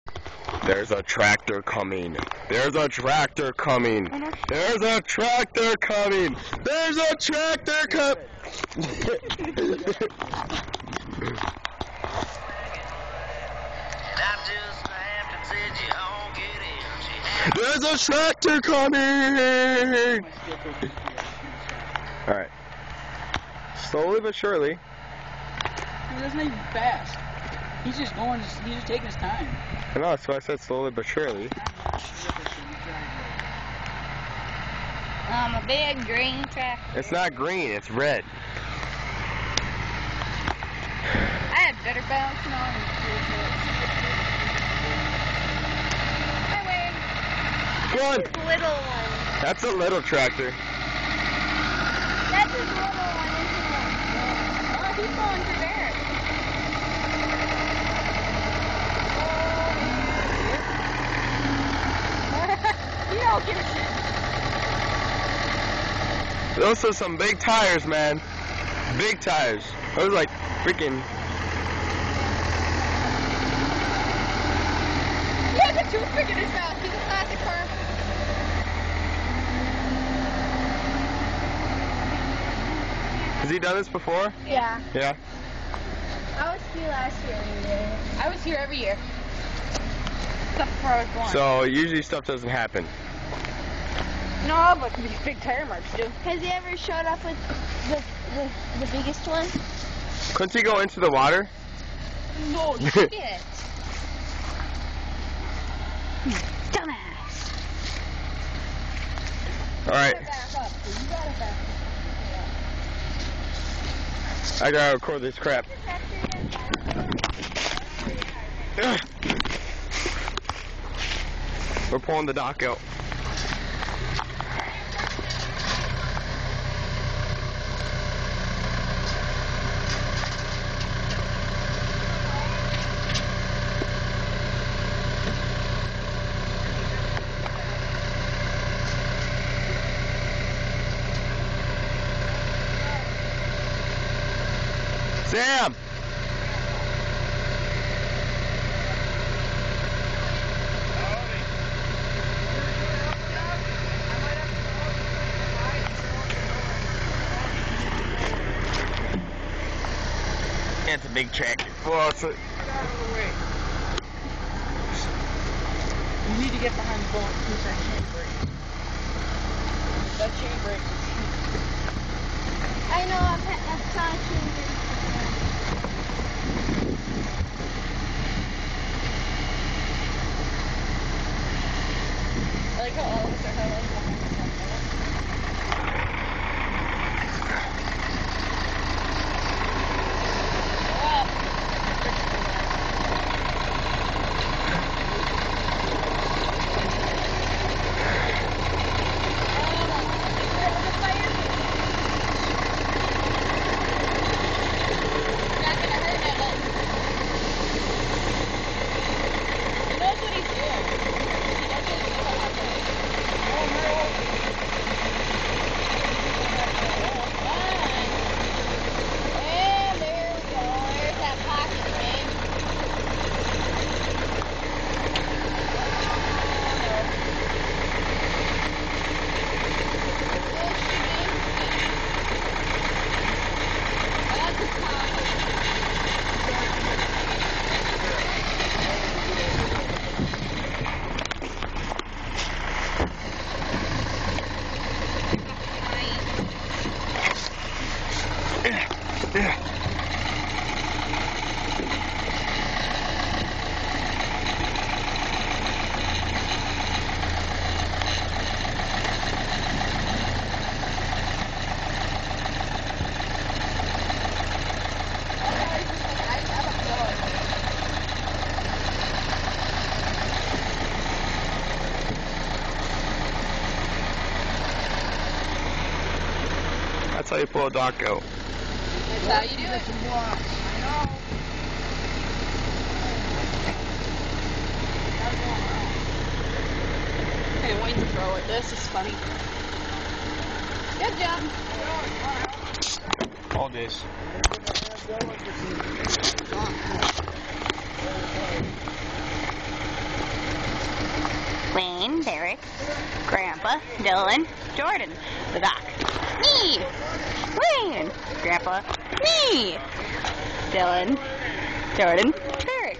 Redneck Dock Pull **FUNNY** (Tractor Pull)
The redneck dock pull. One of the most genius ideas ever.

WARNING: Do not try this at home. Done by professionals. Thank you.